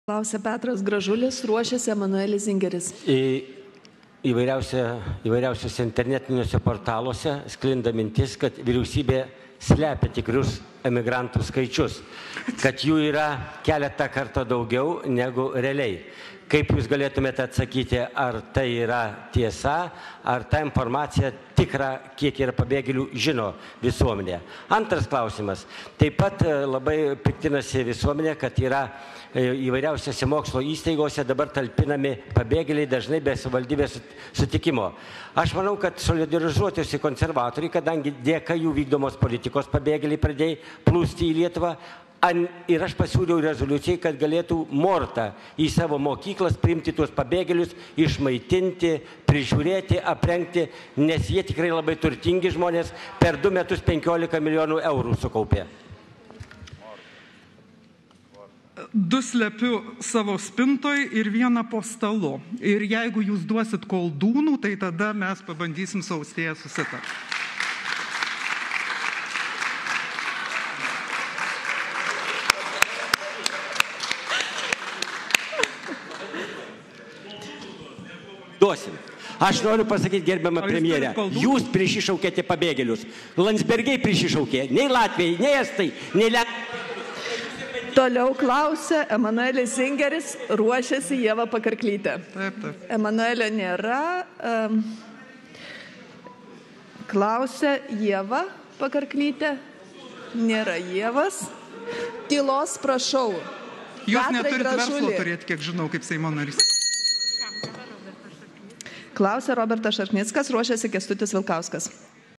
P. Gražulis: Šimonytė slepia tikrus imigrantų skaičius...? 🤣
Ištraukos laikas: 01:21:00